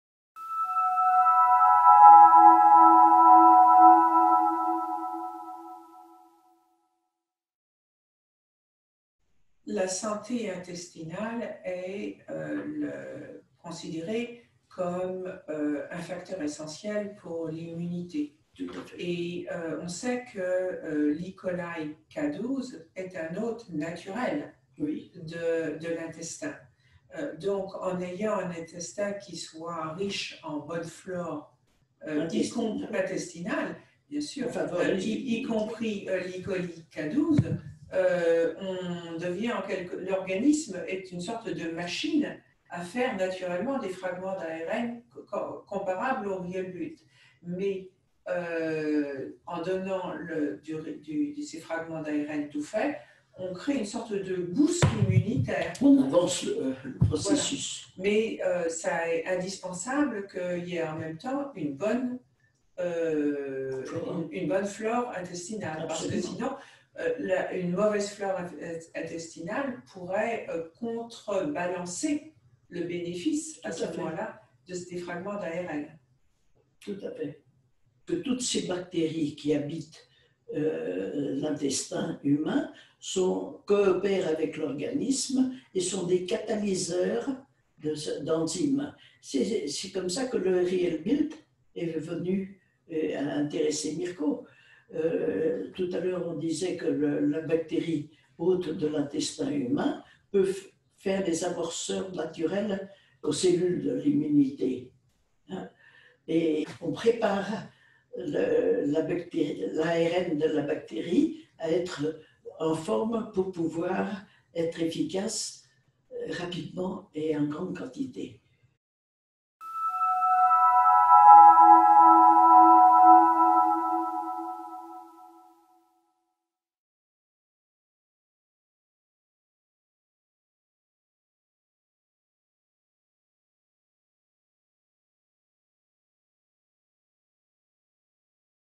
Entretien entre Sylvie et Monique Beljanski: Fragments d'ARN et microbiome
Transcription :

Sylvie : La santé intestinale est considérée comme un facteur essentiel pour l'immunité. Et on sait que l'E-coli K-12 est un hôte naturel de l'intestin. Donc en ayant un intestin qui soit riche en bonne flore intestinale y compris en E-coli K-12, l'organisme est une sorte de machine à faire naturellement des fragments ARN comparables au RealBuild®. En donnant ces fragments d'ARN "tout faits", on crée une sorte de "boost immunitaire".

Monique : On avance le processus

Sylvie : C'est indispensable qu'il y ait en même temps une bonne flore intestinale. Une mauvaise flore intestinale pourrait contrebalancer le bénéfice de ces fragments d'ARN.

Monique : Tout à fait. Toutes ces bactéries qui habitent l'intestin humain coopèrent avec l'organisme et sont des catalyseurs d'enzymes. C'est comme cela que le RealBuild® a intéressé Mirko Beljanski. Tout a l'heure on disait que la bactérie hôte de l'intestin humain peux faire des amorceurs naturels aux cellules de l'immunité. On prépare l'ARN de la bactérie à être en forme pour pouvoir être efficace rapidement et en grande quantité.